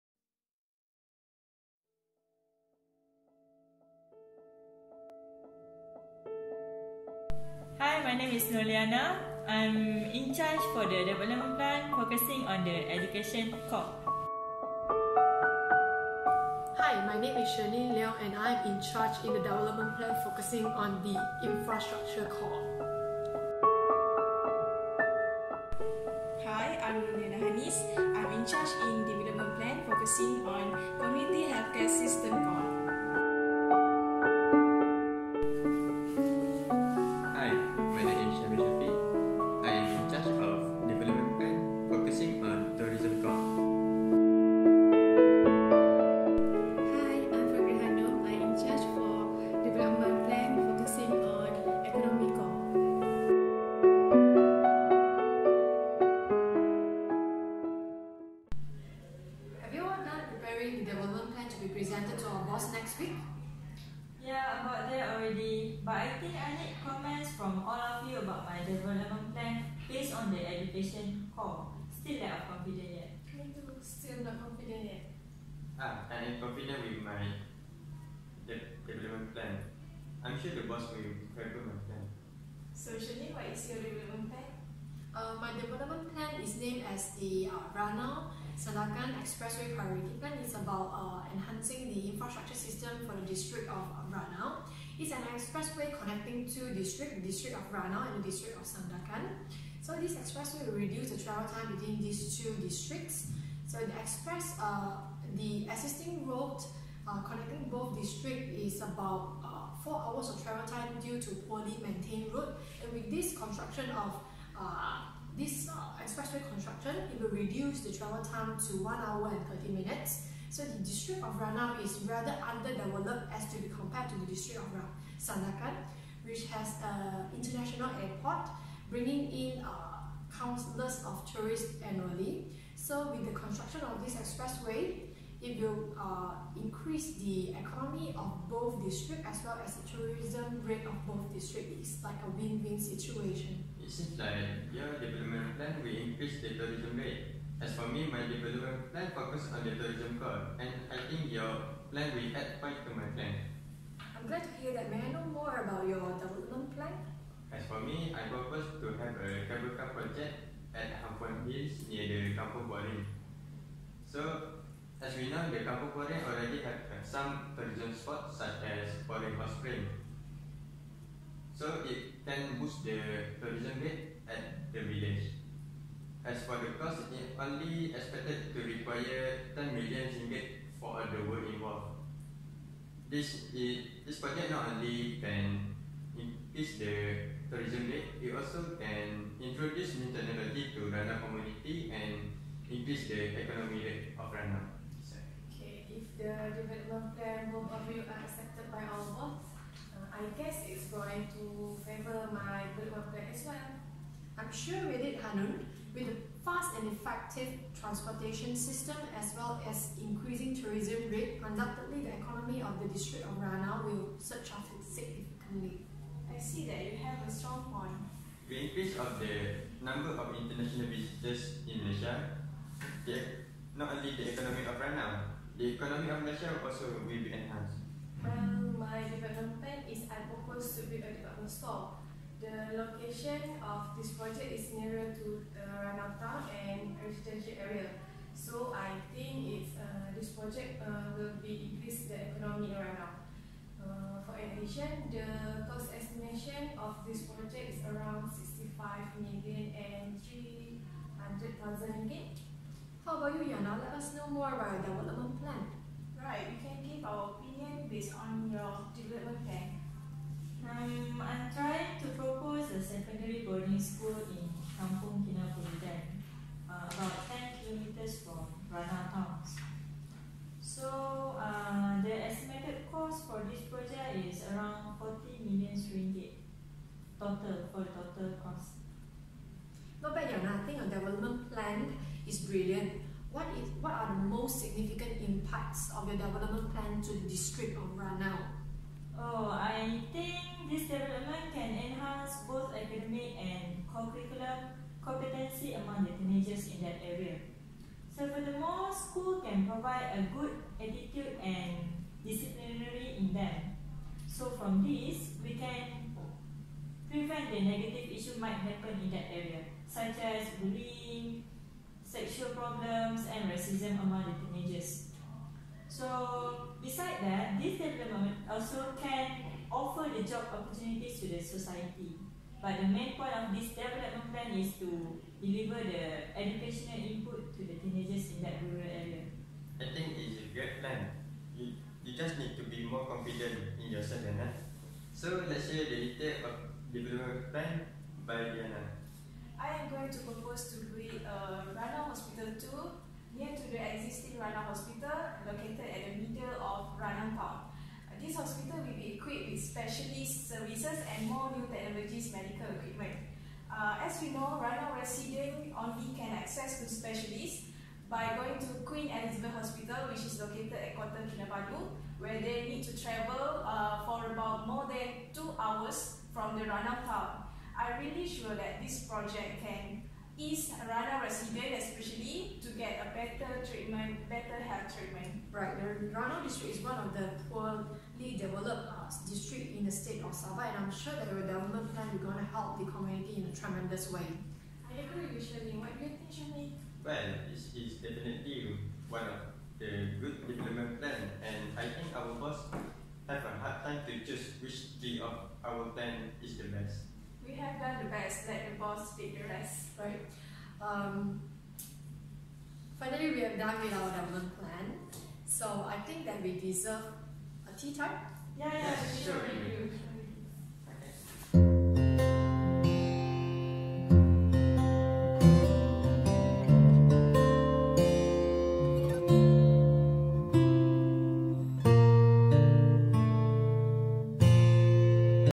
BAA 4222 (ENGINEERS IN SOCIETY) GROUP 17 SEM 1 2018/2019
The district of Ranau was selected to be developed based on five cores, Infrastructure, Education, Tourism, Economy and Community Health Care System. Five development plans were proposed for each core and these development plans were closely proposed to curtail the needs of the people of Ranau. Also, the development plans are expected to enhance the life of Ranau people from various outlooks. So many districts in the state of Sabah, district of Ranau was picked for all five development plans because the district of Ranau is one of the least developed district in the state of Sabah. We noted that with the unbalanced rate of development in the state of Sabah is causing the people to have communication gap. To remedy this situation, five development plans were proposed, each serves to flourish the district of Ranau in different ways.

(Special thanks to our lecturer Dr Mohd Irwan Bin Adiyanto for guiding us throughout the semester)